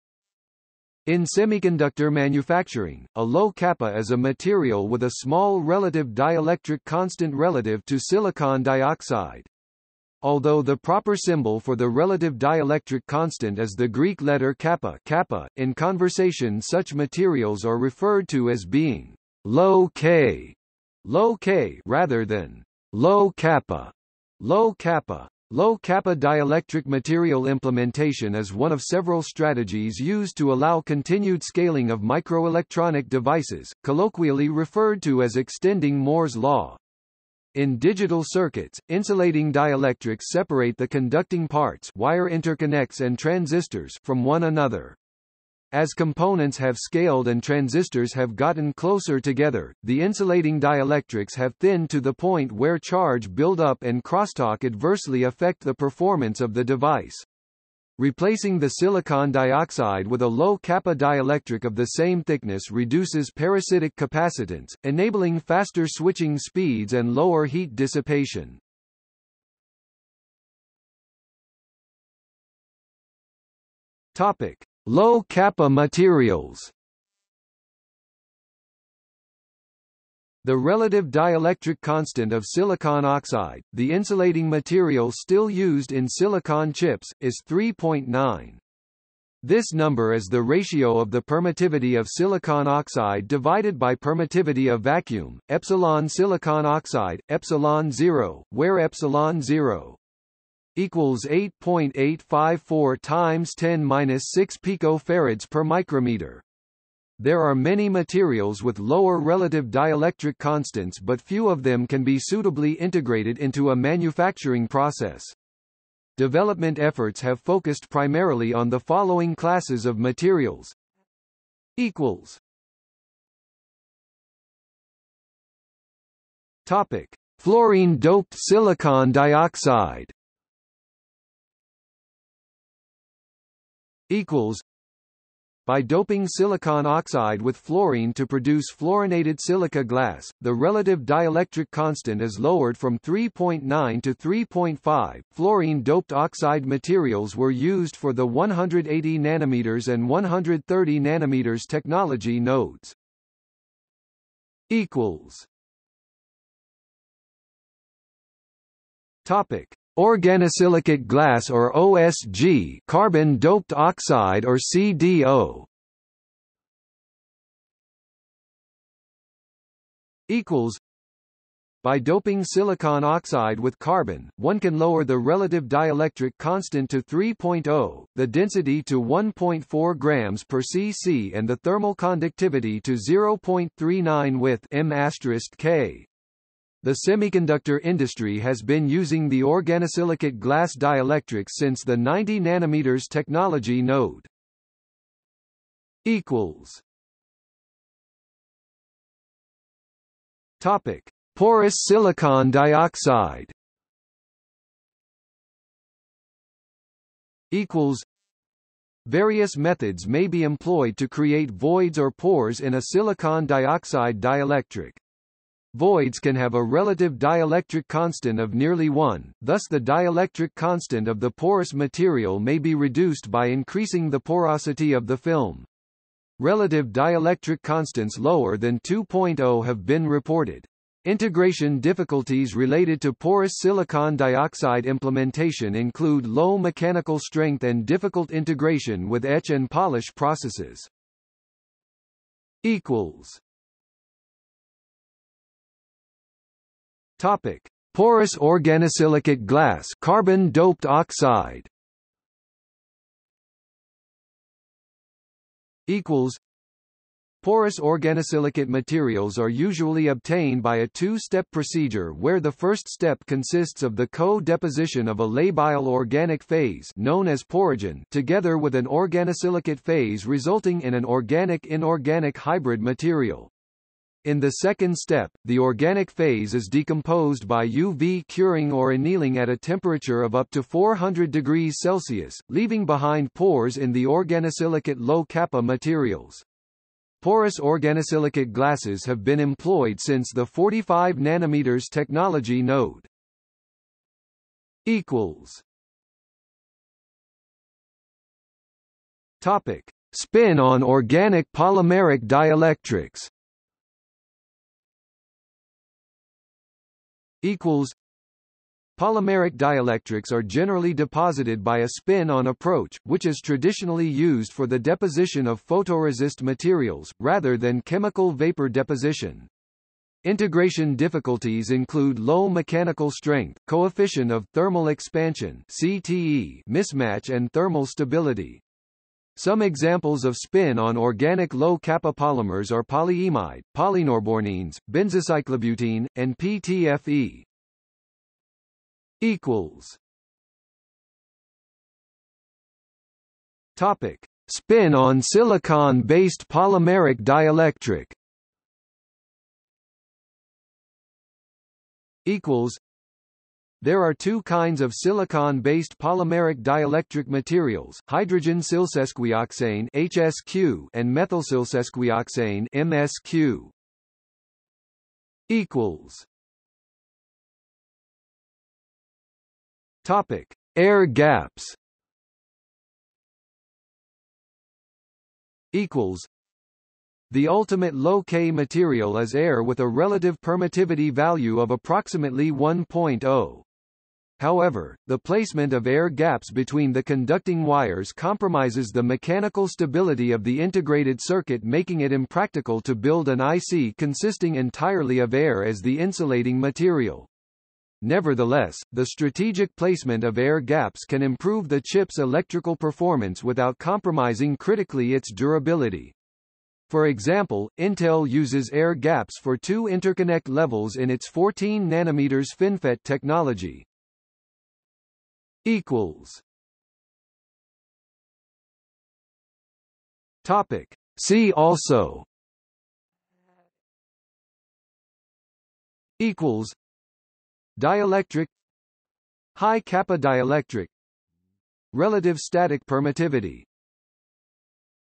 Low-k dielectric | Wikipedia audio article
00:01:10 1 Low-κ materials
00:01:40 1.1 Fluorine-doped silicon dioxide
00:02:03 1.2 Organosilicate glass or OSG (Carbon-doped oxide or CDO)
00:02:14 1.3 Porous silicon dioxide
00:02:35 1.4 Porous organosilicate glass (carbon-doped oxide)
00:02:50 1.5 Spin-on organic polymeric dielectrics
00:03:17 1.6 Spin-on silicon based polymeric dielectric
00:03:27 1.7 Air gaps
00:04:03 2 See also
00:04:15 3 References
00:04:55 4 External links
00:05:07 Polymeric dielectrics are generally deposited by a spin-on approach, which is traditionally used for the deposition of photoresist materials, rather than chemical vapor deposition. Integration difficulties include low mechanical strength, coefficient of thermal expansion (CTE) mismatch and thermal stability. Some examples of spin-on organic low-κ polymers are polyimide, polynorbornenes, benzocyclobutene, and PTFE.
00:05:40 Spin-on silicon based polymeric dielectric
00:05:52 There are two kinds of silicon based polymeric dielectric materials, hydrogen silsesquioxane (HSQ) and methylsilsesquioxane (MSQ).
00:06:06 Air gaps
00:06:15 The ultimate low-k material is air with a relative permittivity value of ~1.0. However, the placement of air gaps between the conducting wires compromises the mechanical stability of the integrated circuit making it impractical to build an IC consisting entirely of air as the insulating material. Nevertheless, the strategic placement of air gaps can improve the chip's electrical performance without compromising critically its durability. For example, Intel uses air gaps for two interconnect levels in its 14 nm FinFET technology.
00:06:56 See also

- Socrates

SUMMARY
In semiconductor manufacturing, a low-κ is a material with a small relative dielectric constant relative to silicon dioxide. Although the proper symbol for the relative dielectric constant is the Greek letter κ (kappa), in conversation such materials are referred to as being "low-k" (low-kay) rather than "low-κ" (low-kappa). Low-κ dielectric material implementation is one of several strategies used to allow continued scaling of microelectronic devices, colloquially referred to as extending Moore's law. In digital circuits, insulating dielectrics separate the conducting parts (wire interconnects and transistors) from one another. As components have scaled and transistors have gotten closer together, the insulating dielectrics have thinned to the point where charge build up and crosstalk adversely affect the performance of the device. Replacing the silicon dioxide with a low-κ dielectric of the same thickness reduces parasitic capacitance, enabling faster switching speeds and lower heat dissipation.